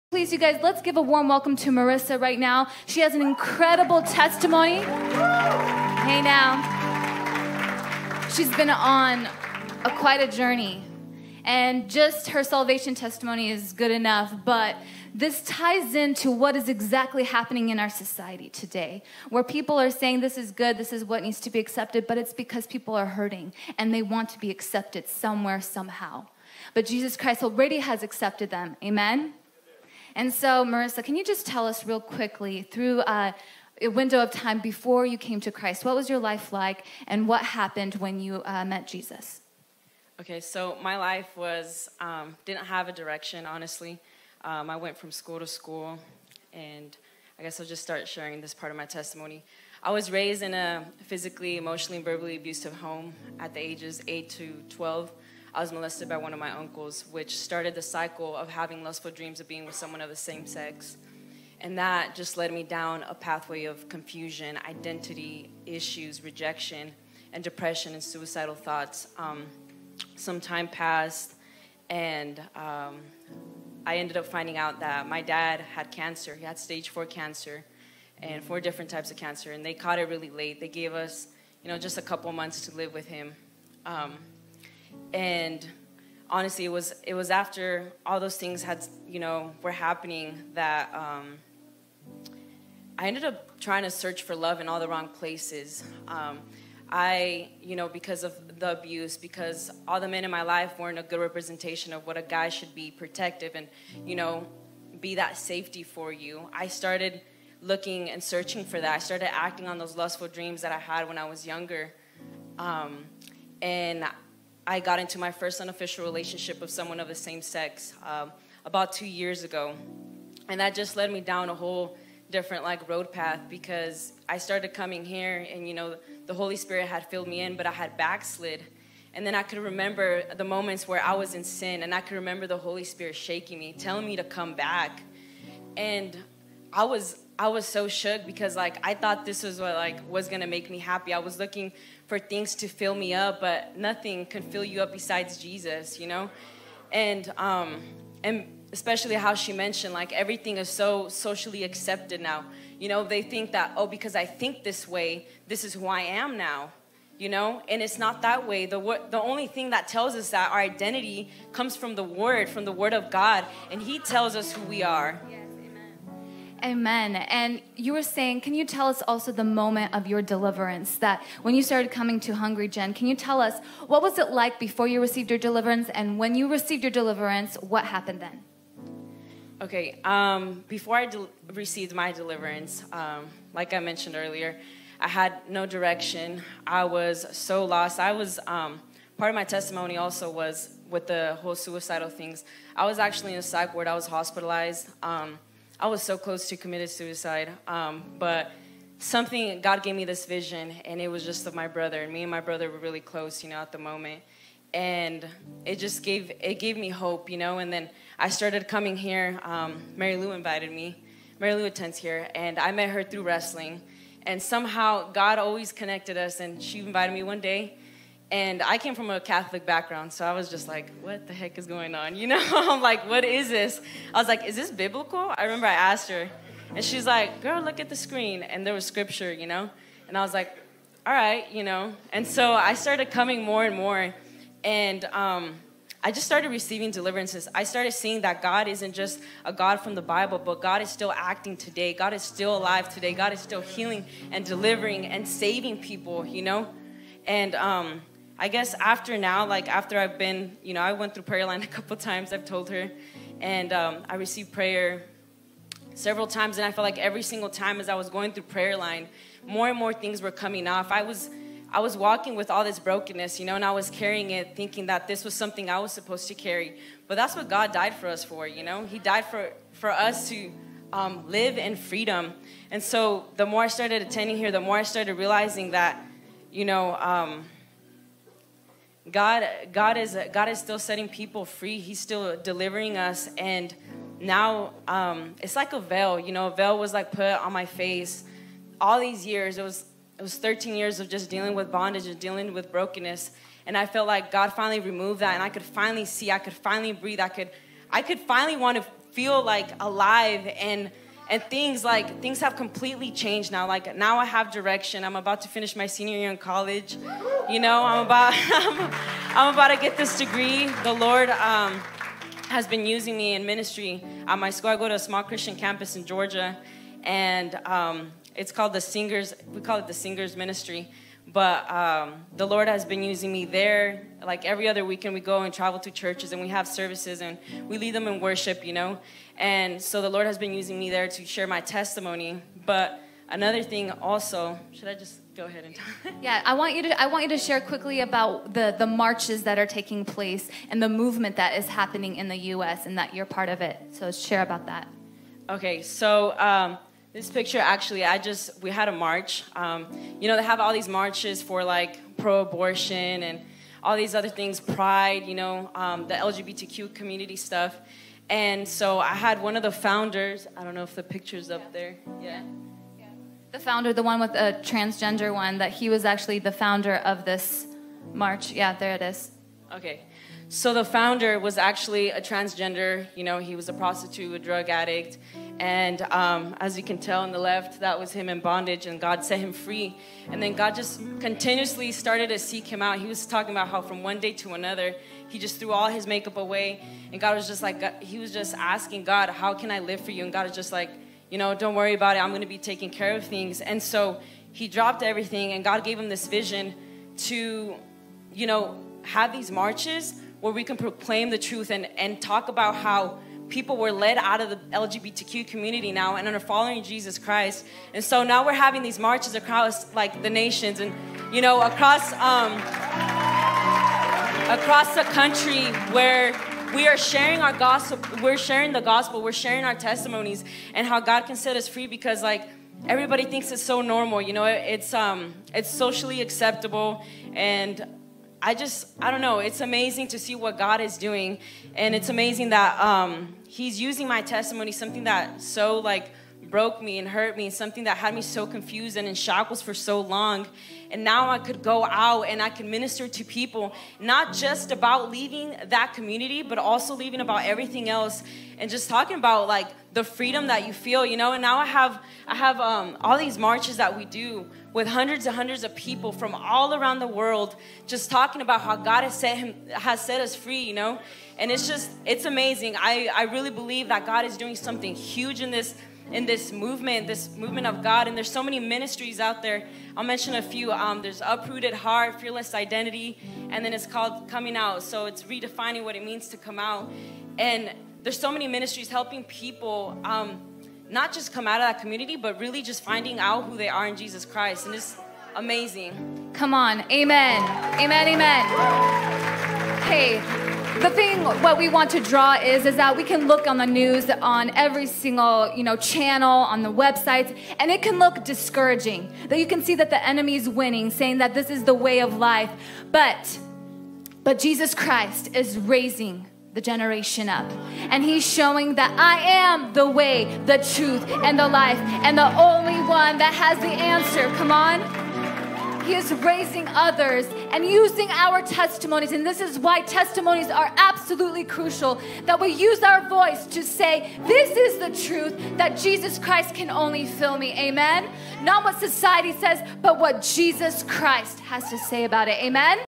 마리사의 간증 [헝그리제너레이션]
가족의 성적 학대와 폭력으로 멍든 삶에서
구원해주신 하나님

순종하는 삶의 시작을 함께 해주셔서 감사합니다.
주님의 말씀을 전하는데 큰 힘이 됩니다.